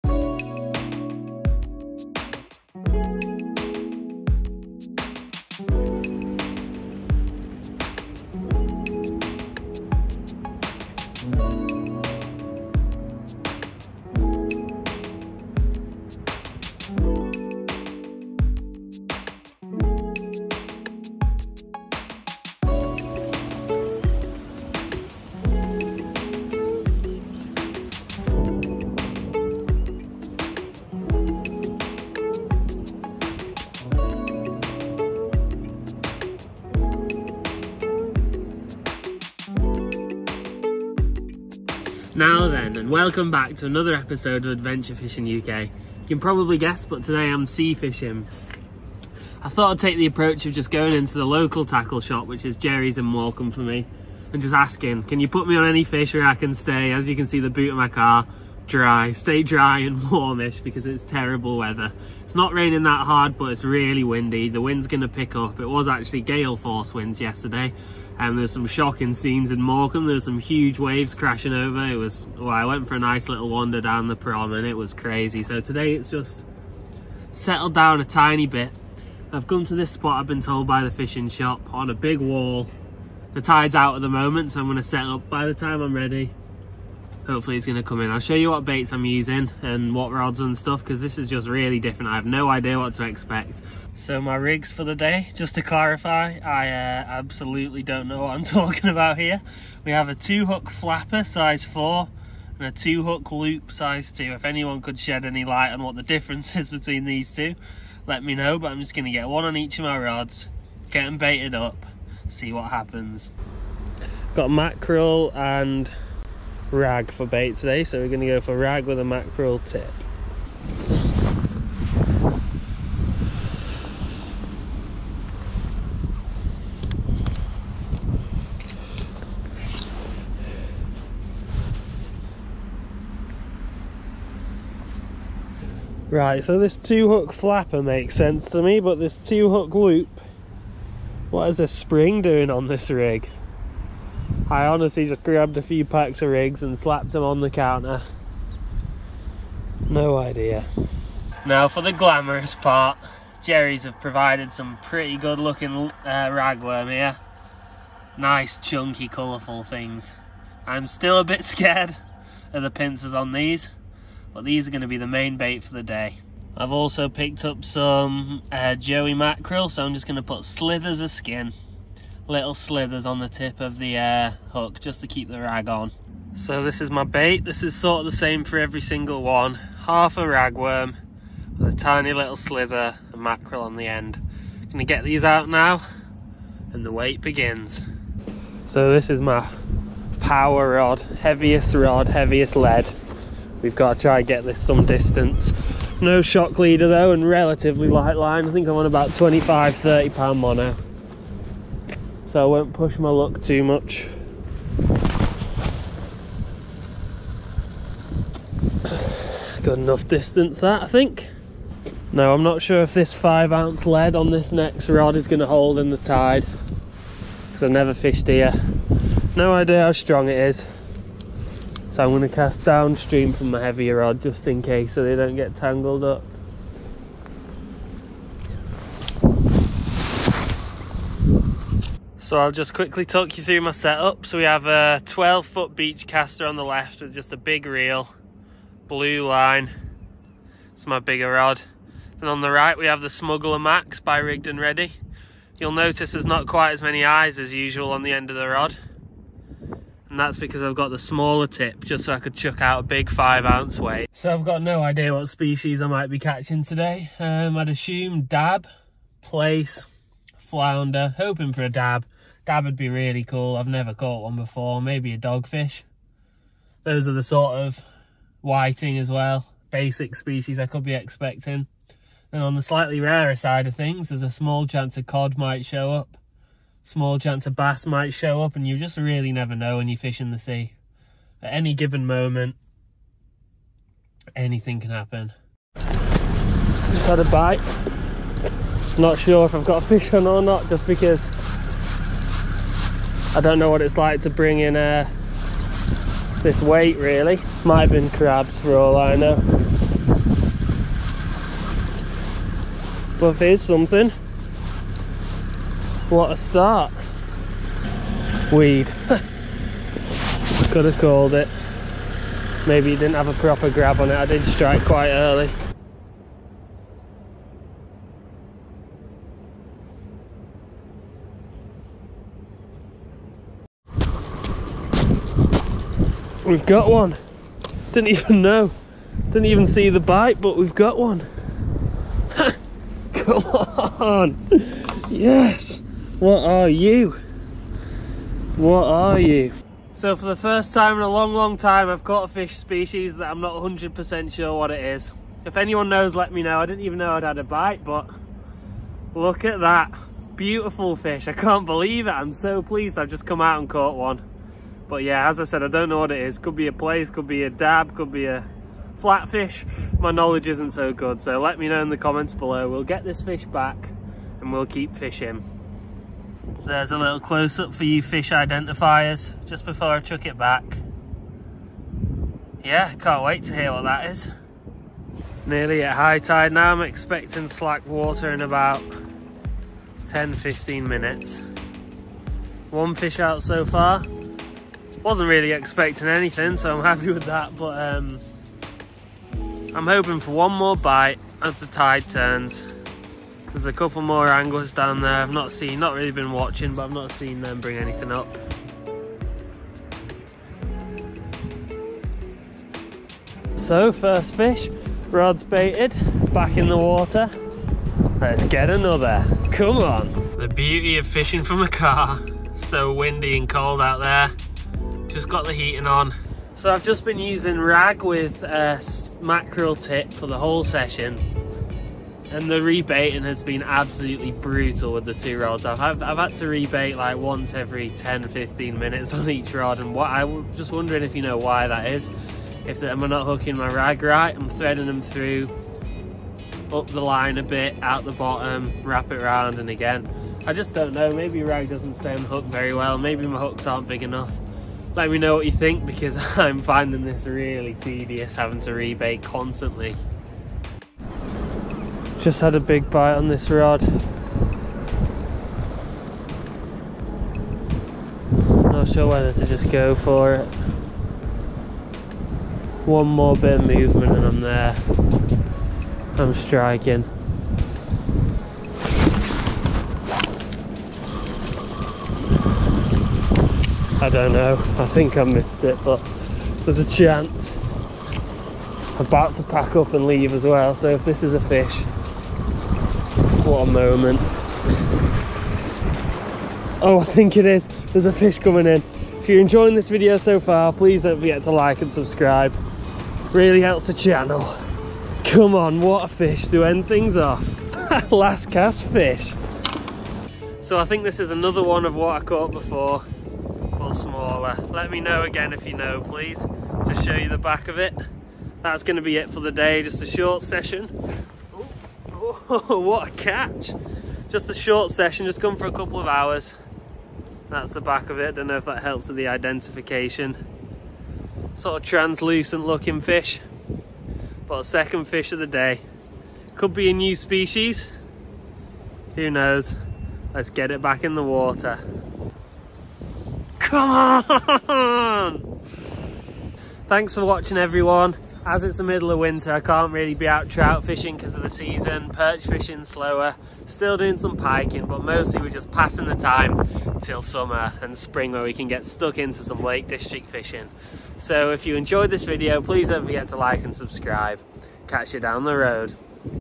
Morecambe Bay Sea Fishing - ❄️ Winter on HEYSHAM NORTH WALL
Winter sea fishing at Heysham for anything that bites. Not going to pretend I know what i'm doing here, I don't often sea fish unless it's with lures. However, it was great to get a result and potentially a new species - which i now believe to be dab (I could be wrong). Soon the videos on this channel will be a lot more lake district orientated and i will be back out lure fishing for trout and perch in the mountains and lakes as soon as the season is over! Thanks to everyone who watches the videos, this winter i've tried to fish some different venues and some different methods, like here, just to keep things interesting.

See you for the next video!